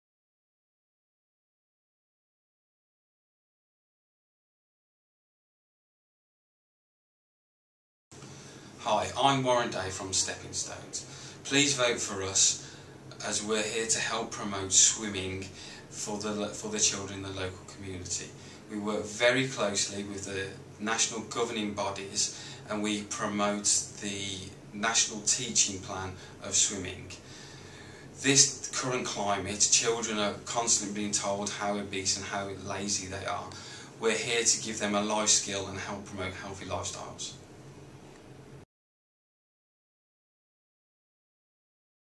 Stepping Stones - Swimming lessons
Amington Participatory Budgeting - Your Voice, Your Choice 2012
Children will go through different classes where they will learn basic skills -- such as how to enter and exit the water safely -- through to more advanced techniques -- such as how to breathe in the water and perform different strokes. This will promote healthier lifestyles, provide children with a skill for life and develop social skills.